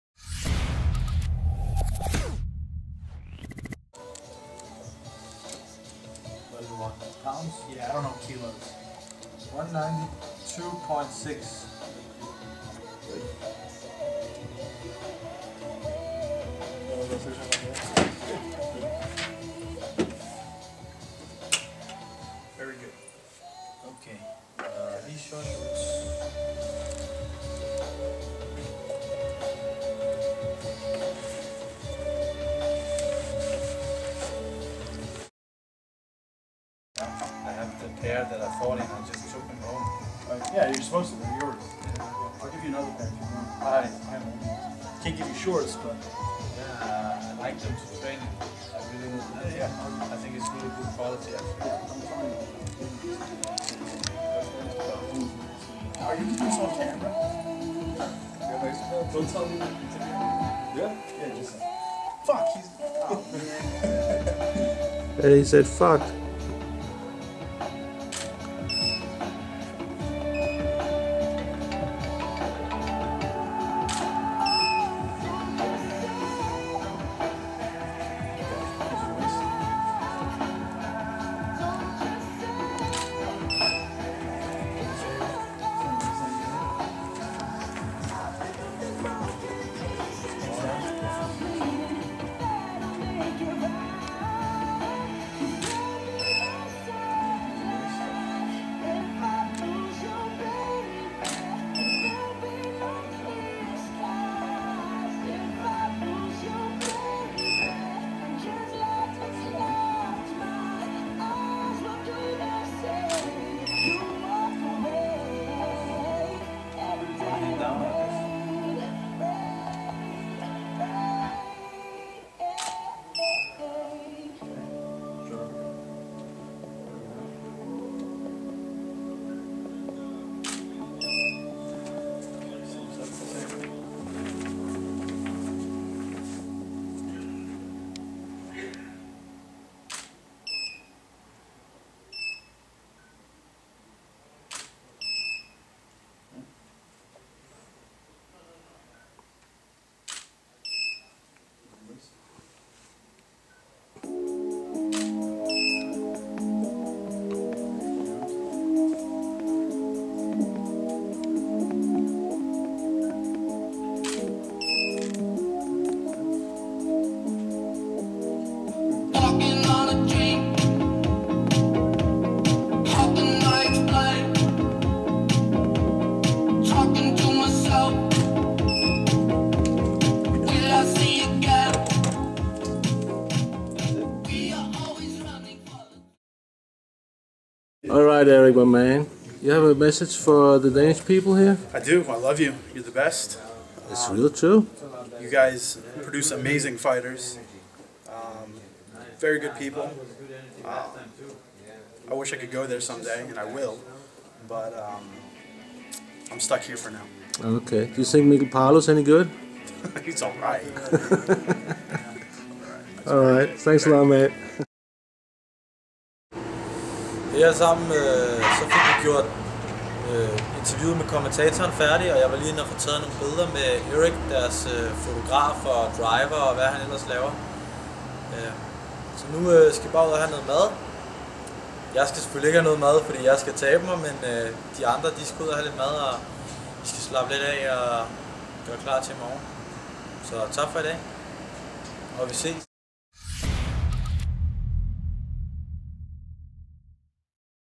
Mikkel Parlo - Bellator vejning og photo shoot
Her kan du komme med om bag kulisserne når Parlo skal vejes og have lavet photo shoot. Ting der også er en del af en professionel MMA fighters hverdag.